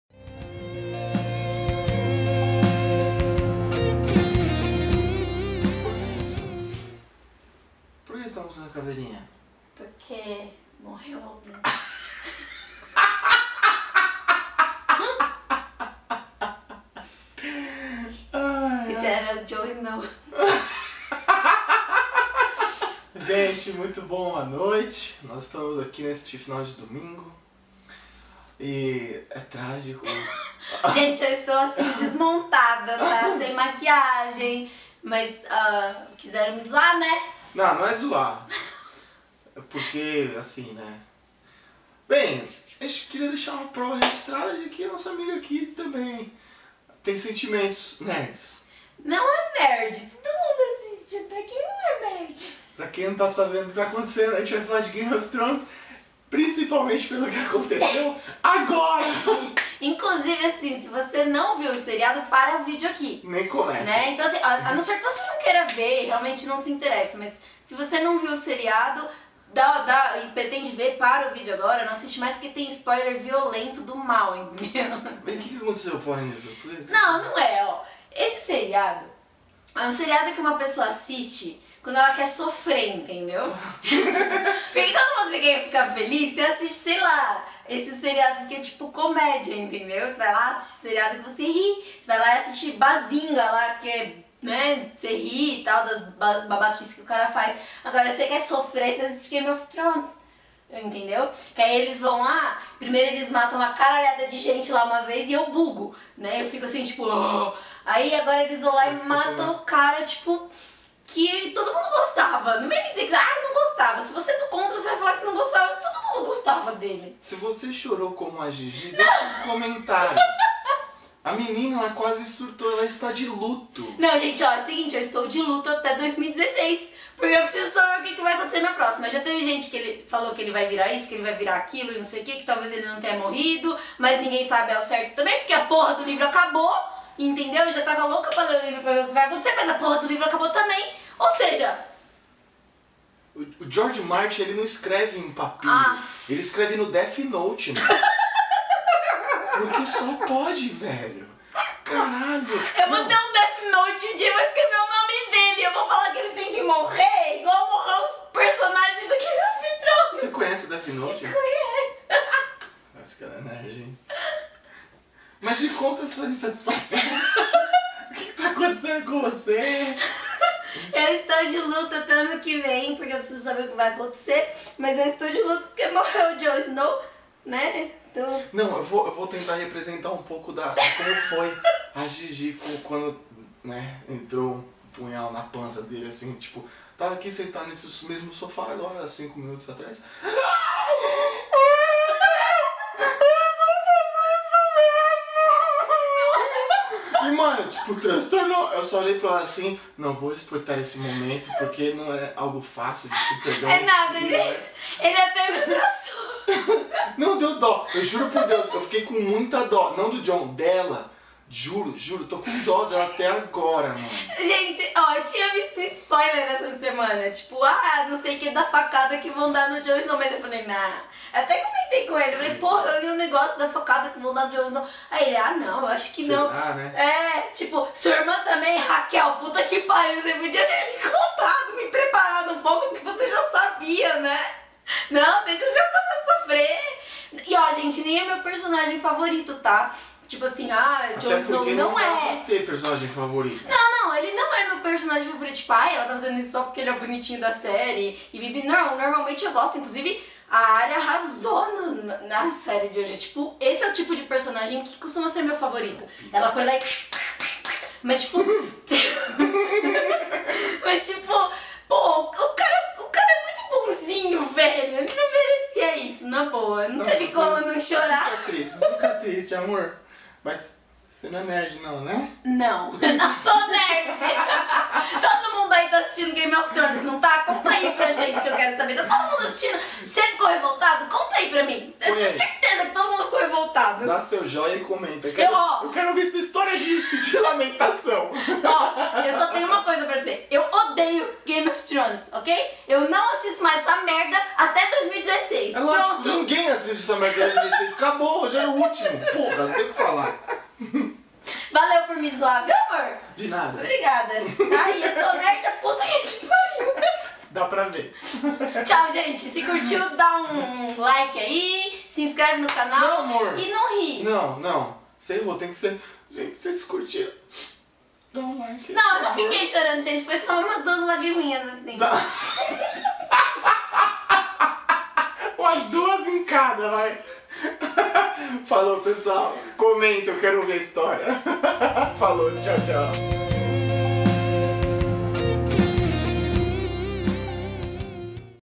EU NÃO SOU NERD #2 - Odeio Game of Thrones (spoiler)
*ATENÇÃO, CONTÉM SPOILERS VIOLENTOS!!*

Sabe quando você tem um namorado que não perde a oportunidade de tirar um sarro do seu sofrimento? Então...